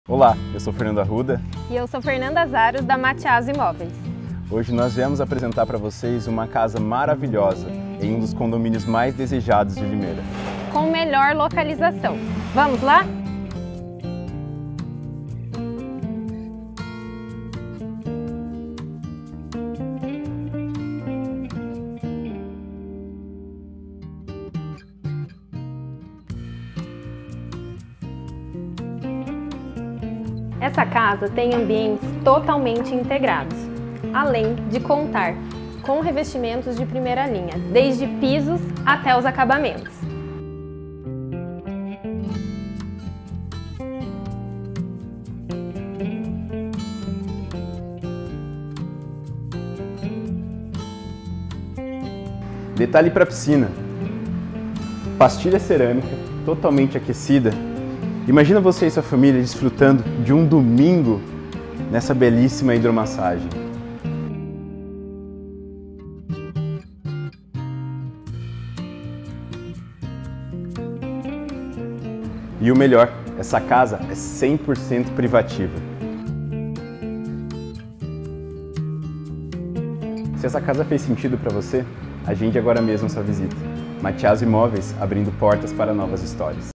CASA À VENDA CONTEMPORÂNEA COM PROJETO INCRÍVEL, NO CONDOMÍNIO TERRAS DE SÃO BENTO II, LIMEIRA/SP
Para você que deseja um imóvel com diferenciais únicos, prático e muito seguro, esta belíssima casa térrea à venda em condomínio é perfeita para você.

Sofisticação, conforto e uma deliciosa área gourmet com piscina aquecida são os destaques dessa casa. Com ambientes amplos banhados à luz natural logo pela manhã, a casa conta com acabamentos e móveis planejados de alto padrão.

O imóvel contém:

- 3 dormitórios, sendo 1 suíte master,
- Sala para 2 ambientes,
- Lavabo,
- Cozinha americana,
- Banheiro social,
- Lavanderia,
- Espaço gourmet,
- Piscina aquecida,
- Esquadrias em pvc,
- Móveis planejados,
- Garagem para 2 veículos.

Ref. 6455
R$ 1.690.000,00

As melhores casas em condomínio de Limeira/SP você encontra na Mattiazzo Imóveis.

Venha conversar com nossos corretores.

Mattiazzo Imóveis
CRECI 21832J